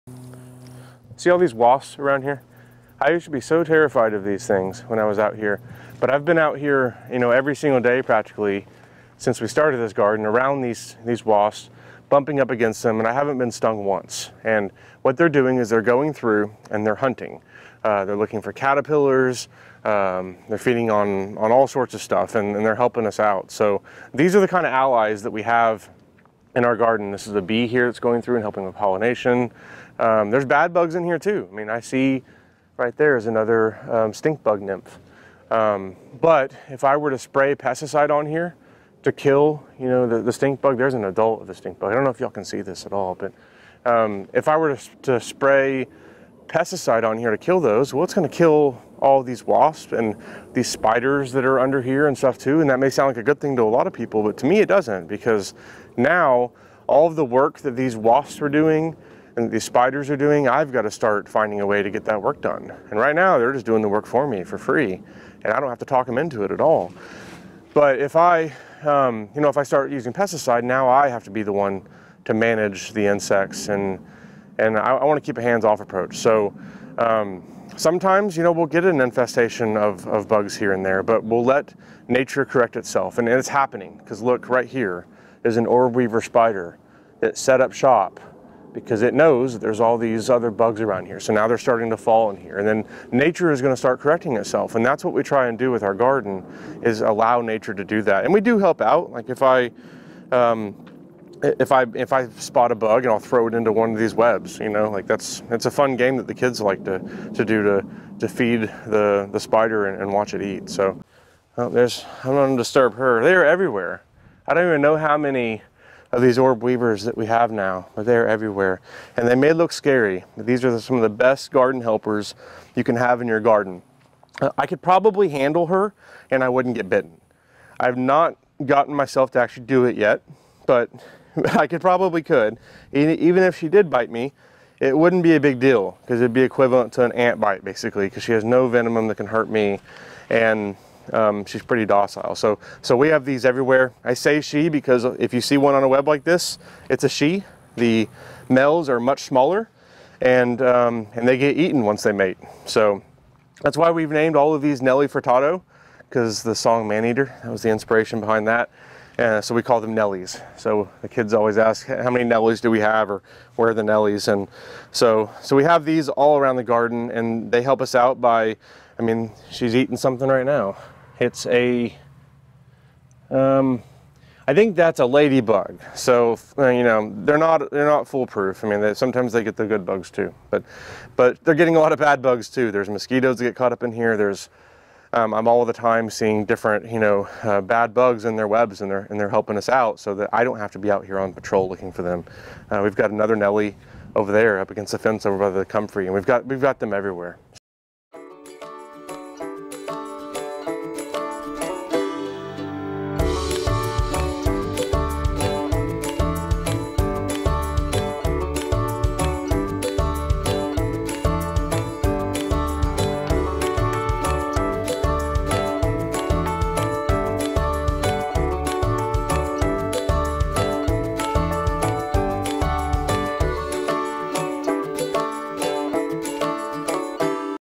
Garden Allies - How Wasps, Bees & Spiders Help Us & Why We Don't Use Pesticide
This video talks about how wasps, bees and spiders help us in the garden, and why we don't use pesticides.

See more videos, blog posts and download our free mobile app @ seedtospoon.net